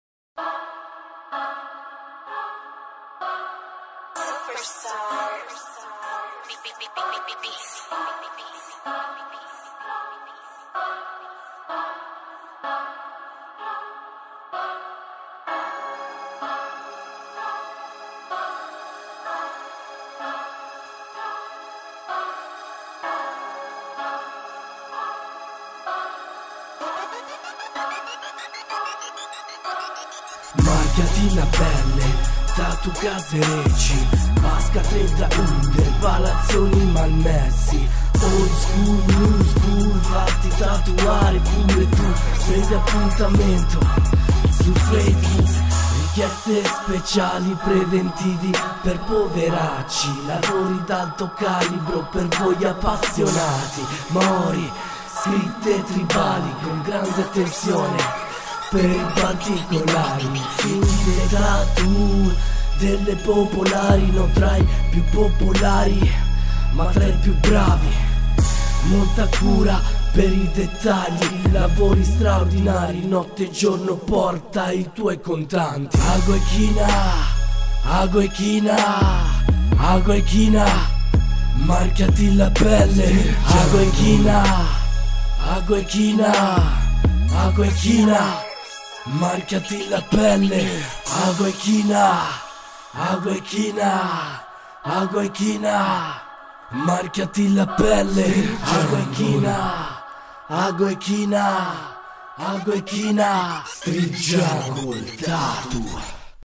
STREET JUNGLE TATTOO doog mc VIDEO
Video dedicato al tatuatore GIULIANO detto anche STREET JUNGLE TATTOO
PAGINA UFFICIALE FACEBOOK STREET JUNGLE TATTOO
VIDEO AUTOPRODOTTO E MONTATO DA DOOG MC Gioventu' Bruciata Crew
CONTATTO FACEBOOK